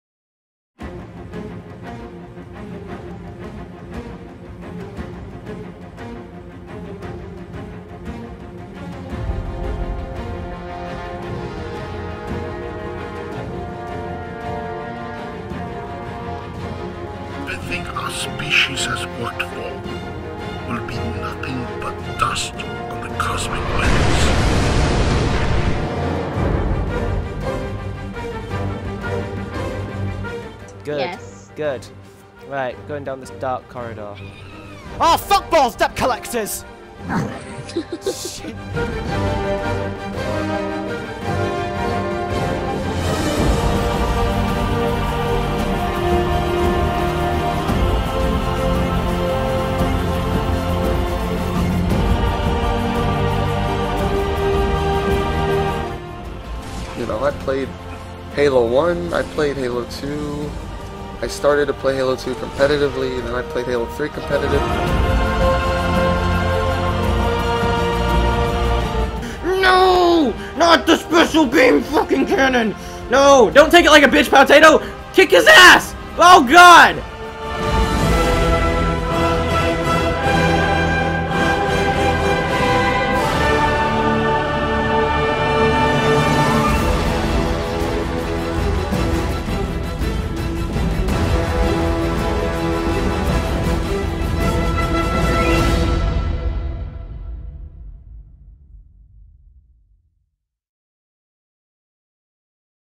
Welcome To The Silence Network
The Silence Network is a united gaming community dedicated to bringing you high quality gaming related content, whether it is an interesting Machinima that explores the values of life, or an opinion you wish to express through the latest Deus Ex game. We are open to community feedback, including suggestions for content that will interest you.